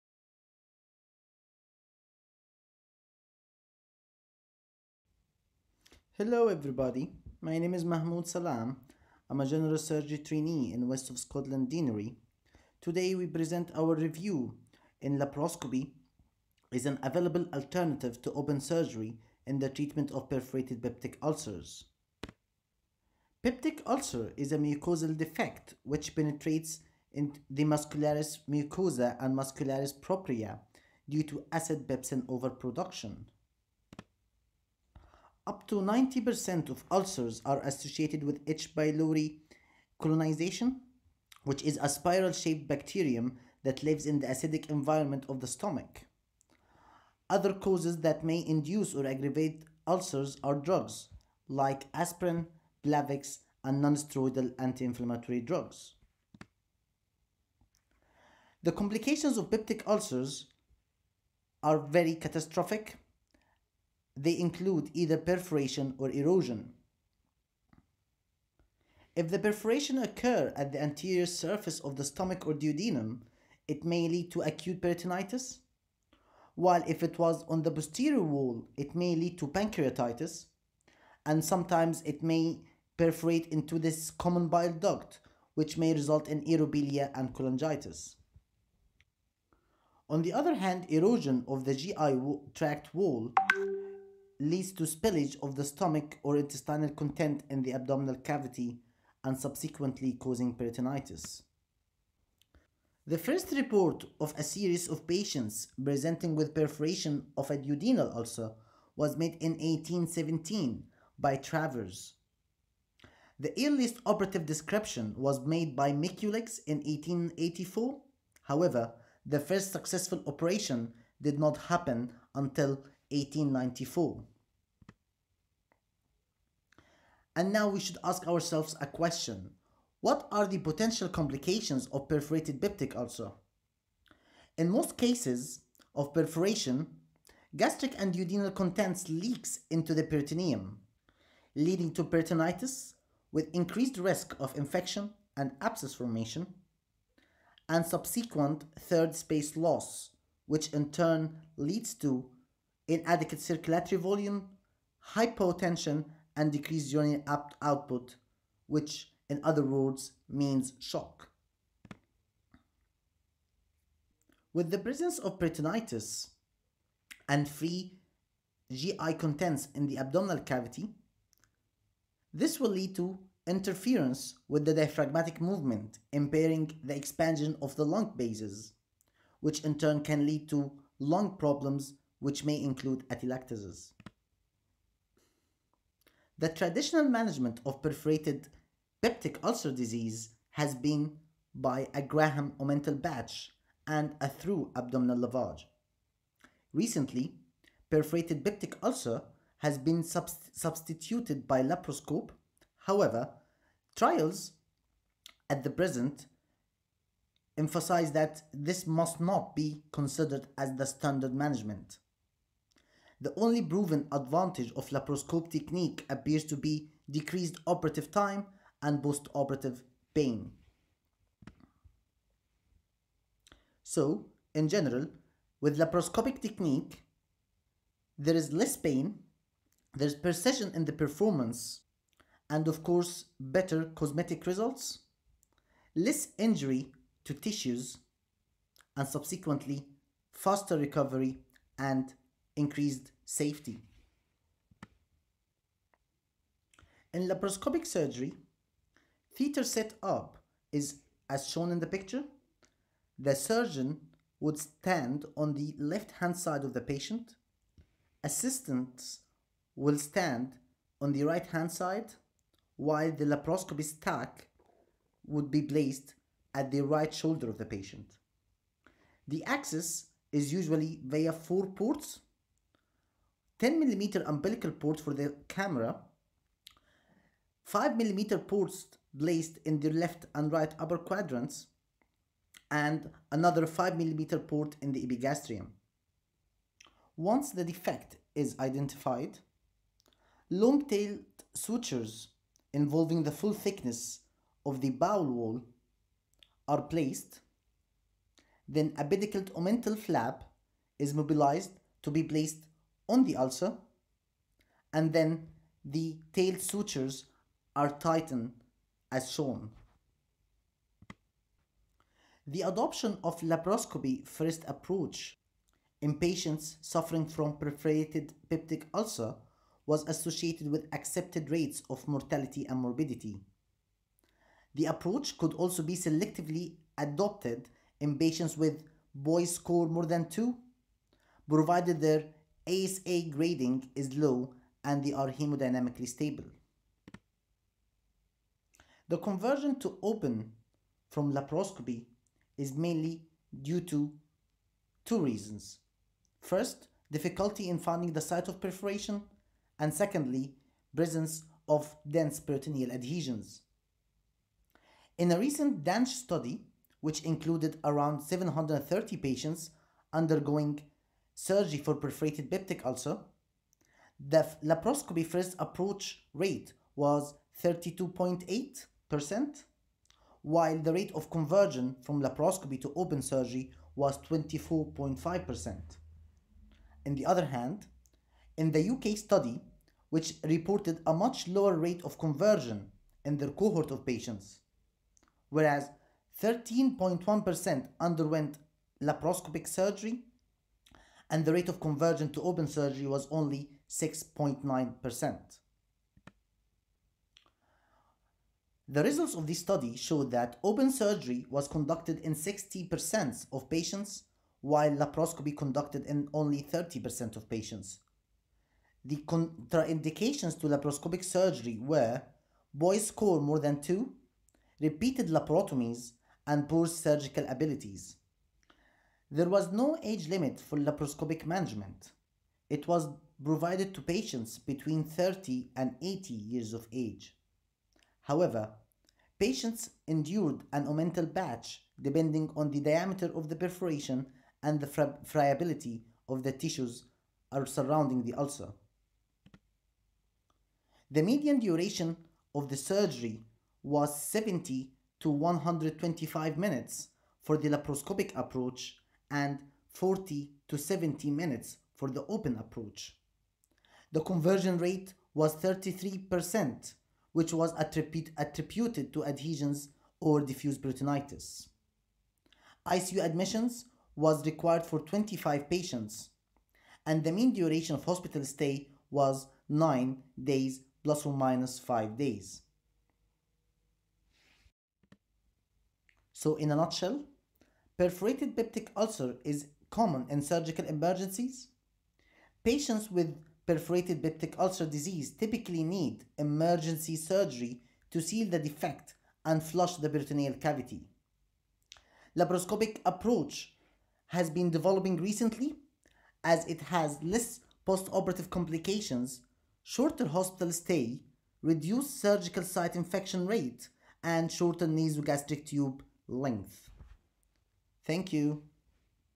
Laparoscopy is an Available Alternative to Open Surgery in the Treatment of Perforated Peptic Ulcers
Authors: Jimmy Mena, Mahmoud Sallam

Abstract: Peptic ulcer disease (PUD), which affects 4 million people worldwide per year, has a perforation incidence of 14 per cent. Perforated gastric ulcers (PPUs) have a high morbidity and mortality. Surgical repair is the standard procedure for PPUs. This literature review provides a comparison of open and laparoscopic procedures as well as the latest updates to laparoscopic repair techniques of PPUs, and their causes, clinical features, clinical examination, laparoscopic and open surgical techniques, complications, and future prospects. is. Extensive literature research is completed and the latest meta-analysis and review on the topic is taken into consideration. There was no significant difference in postoperative mortality between open and laparoscopic treatments in patients with PPU. In addition, laparoscopic repair results in slightly less postoperative pain and less risk of wound infection. Laparoscopic repair may therefore be warranted as the treatment of choice.